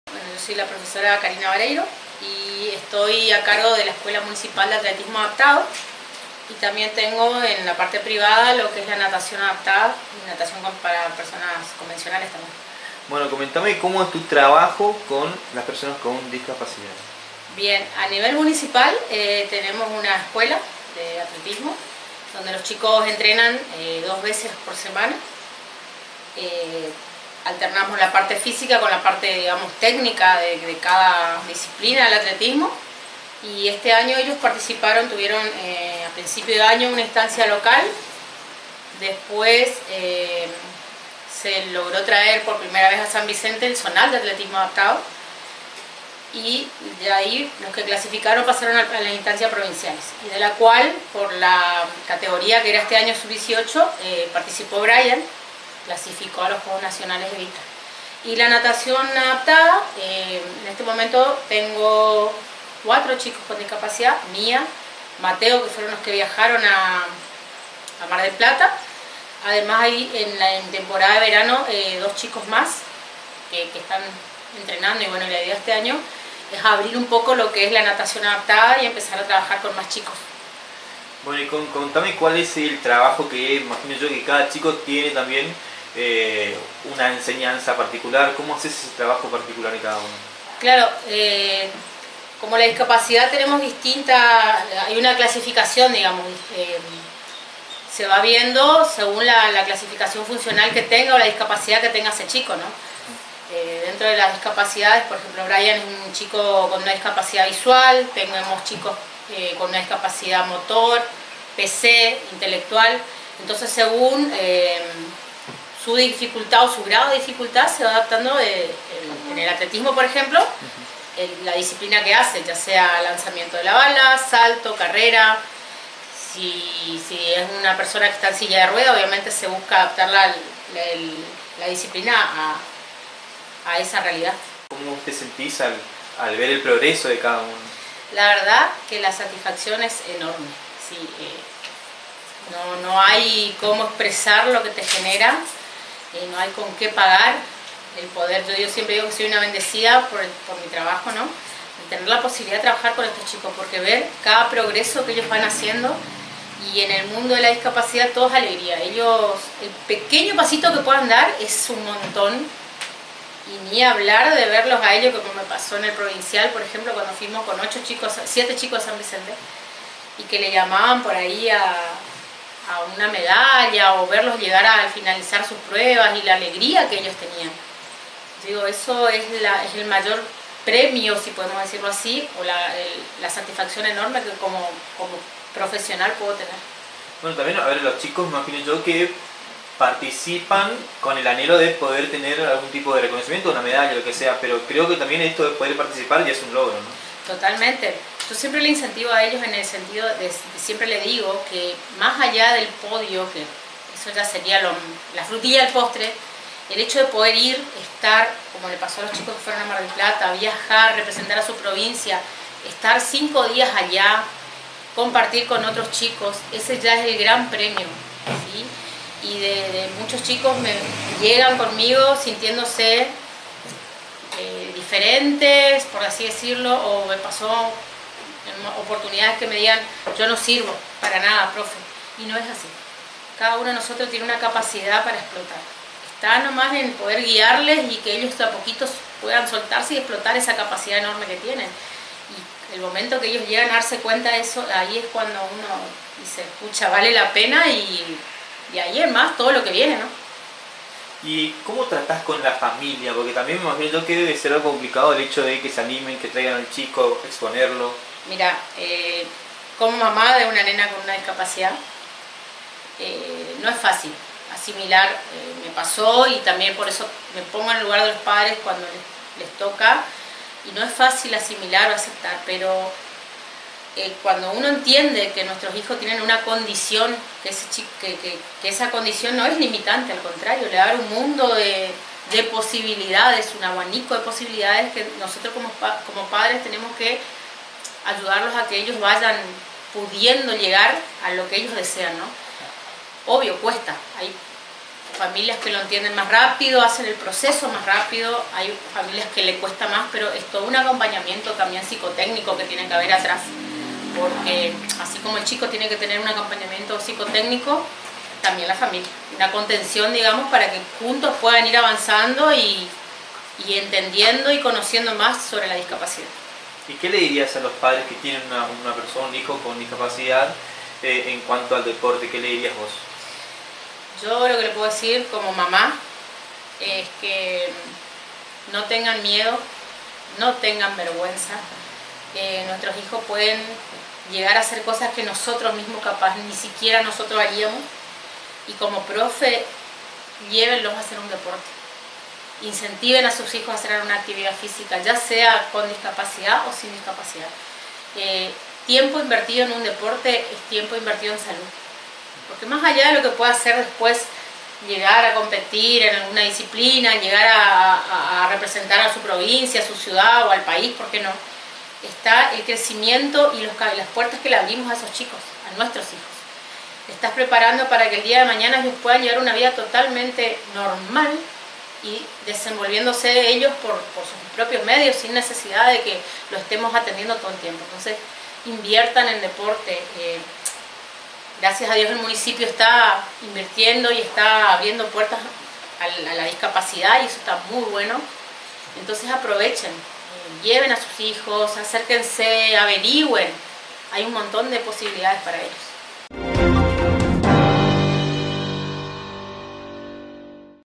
Prof. Karina Bareiro: la importancia del deporte para personas con discapacidad
Canal 7 SVT / Radio 7 FM 90.7 MHz
• Facebook: Canal 7 San Vicente